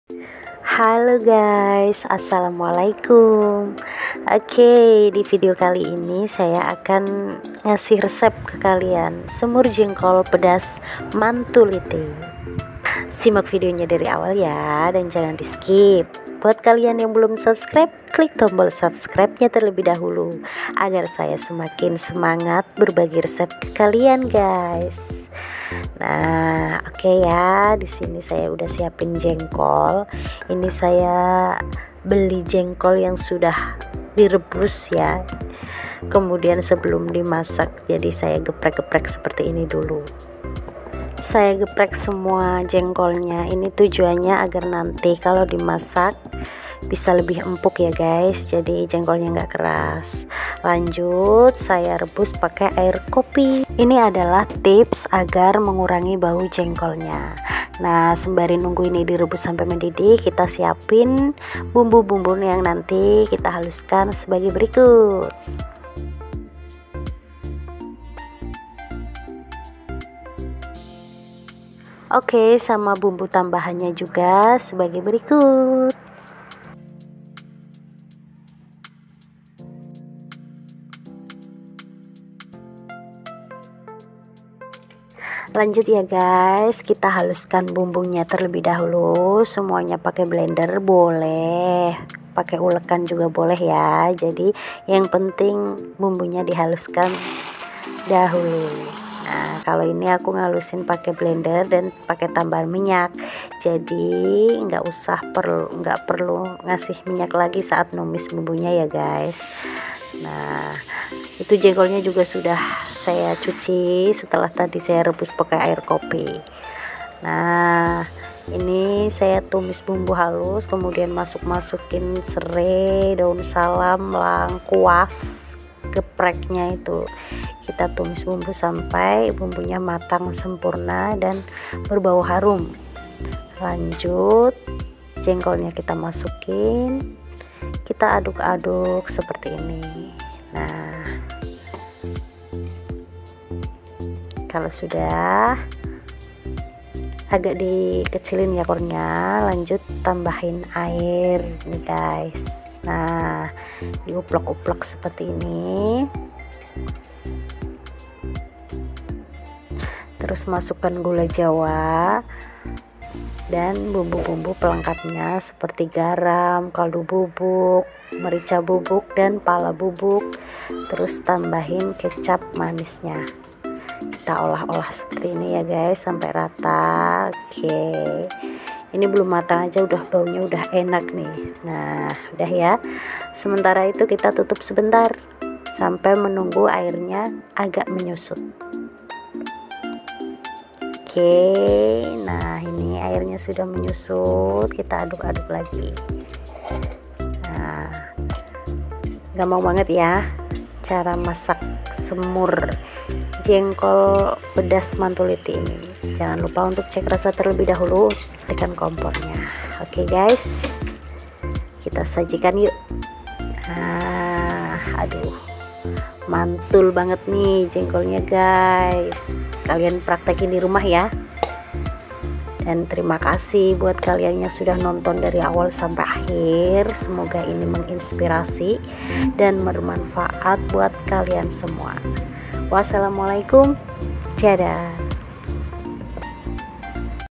SEMUR JENGKOL PEDAS MANTUL ❗BISA DIJADIKAN IDE USAHA JUALAN
Yuk masak semur Jengkol Yang enak dan tidak bau.
Ini adalah resep termudah dan tips agar mengurangi bau jengkol.

Sebwelum dimasak jengkol direbus terlebih dahulu dengan air Kopi hitam. Atau boleh di rendam saja semaleman. Dan paginya bisa langsung di masak.

Bumbu Semur Jengkol
(Banyaknya bisa disesuaikan aja ya, sama banyak jengkol yg kalian masak)

Bawang merah
Bawang putih
Kemiri
Jahe
Kunyit
Lada bubuk
Penyedap bubuk
Garam
Gula jawa
Kcap manis
Pala bubuk
Sedikit air

Selengkapnya sudah ada di video. Selamat menonton, semoga menginspirasi dan bermanfaat